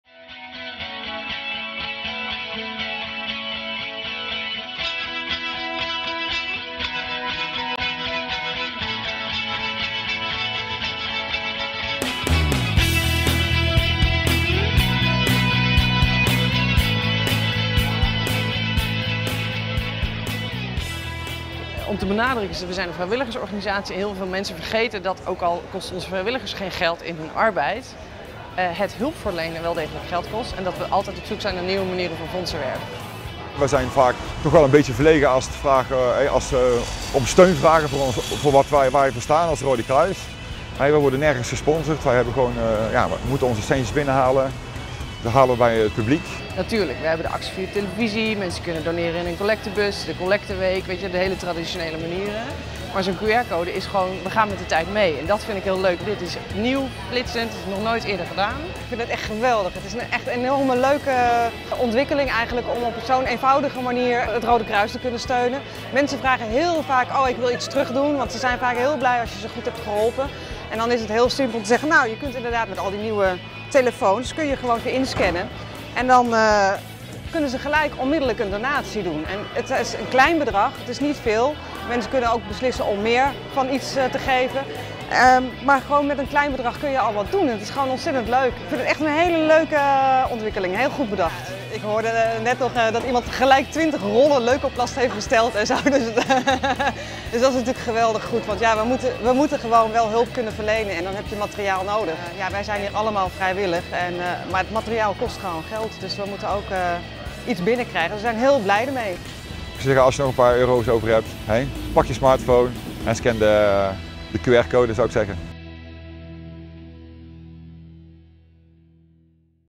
QR Doneren - 4me
Het Nederlandse Rode Kruis start de nieuwe manier van doneren tijdens de Nijmeegse Vierdaagse: QR Doneren met je smart phone.
Wil je doneren? Scan de QR code. De rest wijst zich vanzelf.